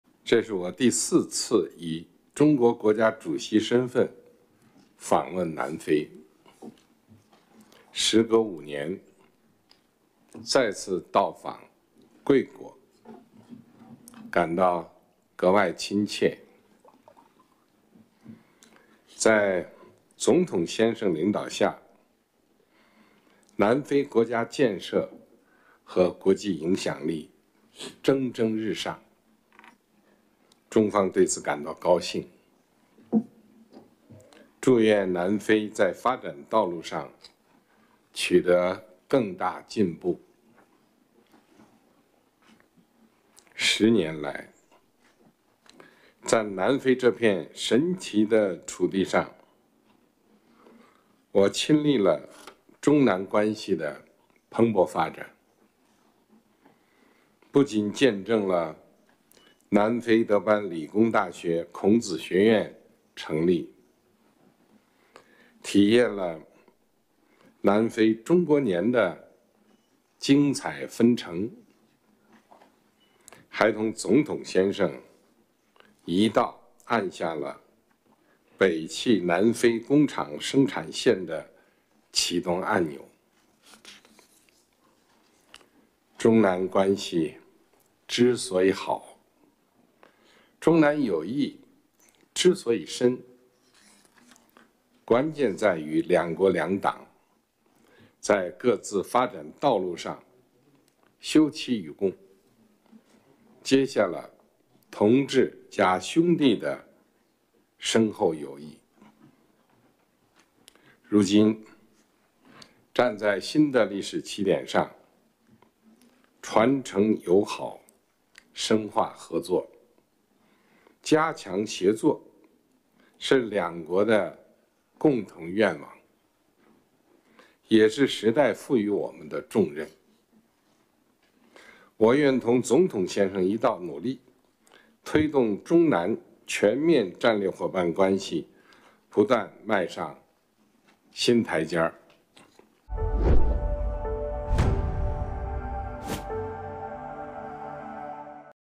习近平同南非总统拉马福萨举行会谈
当地时间8月22日上午，国家主席习近平在比勒陀利亚总统府同南非总统拉马福萨举行会谈。

习近平指出，这是我第四次以中国国家主席身份访问南非。时隔5年再次到访贵国，感到格外亲切。在总统先生领导下，南非国家建设和国际影响力蒸蒸日上。中方对此感到高兴，祝愿南非在发展道路上取得更大进步。10年来，在南非这片神奇的土地上，我亲历了中南关系的蓬勃发展，不仅见证了南非德班理工大学孔子学院成立，体验了南非“中国年”的精彩纷呈，还同总统先生一道按下了北汽南非工厂生产线的启动按钮。

中南关系之所以好、中南友谊之所以深，关键在于两国两党在各自发展道路上休戚与共，结下了同志加兄弟的深厚友谊。如今，站在新的历史起点上，传承友好、深化合作、加强协作是两国的共同愿望，也是时代赋予我们的重任。我愿同总统先生一道努力，推动中南全面战略伙伴关系不断迈上新台阶。